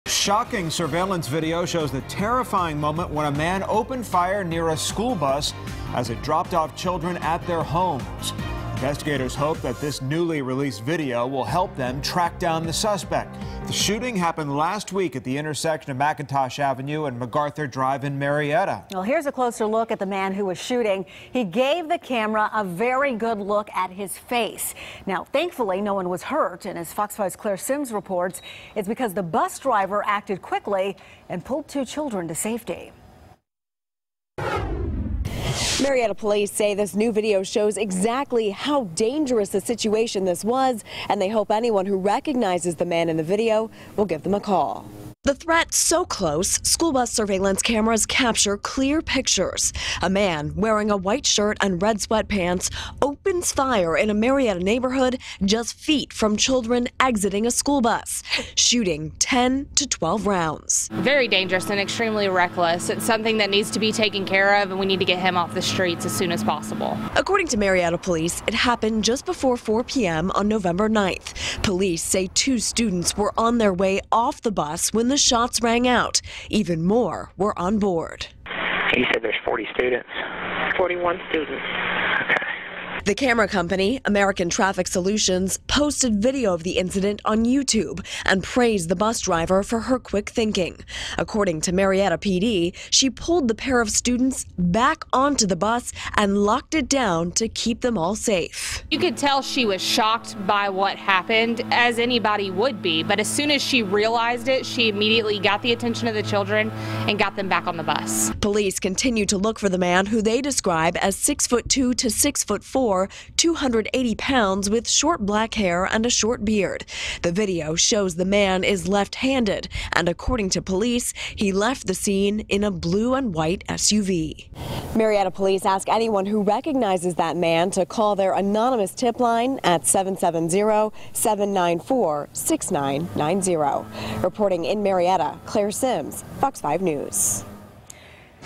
New video shows man opening fire near Marietta school bus
Police hope new surveillance video will help lead them to the man who opened fire near a school bus as children were unloading in Marietta last week.

On November 9, police said two students were attempting to exit the school bus, which was stopped on McIntosh Avenue and McArthur Drive. The bus driver reportedly saw the man firing shots toward a home directly in front of the bus. The driver then grabbed the two children, secured the bus doors and called 911.

Wednesday, American Traffic Solutions, a camera company, uploaded new surveillance video of the incident to YouTube. The cameras on the school bus captured a man firing several rounds just feet from school children making their way home.

Both the company and police praised the bus driver for her quick action.

"You could tell she was shocked by what happened, as anybody would be, but as soon as she realized it, she immediately got the attention of the children and got them back on the bus," said Marietta Police Officer Brittany Wallace.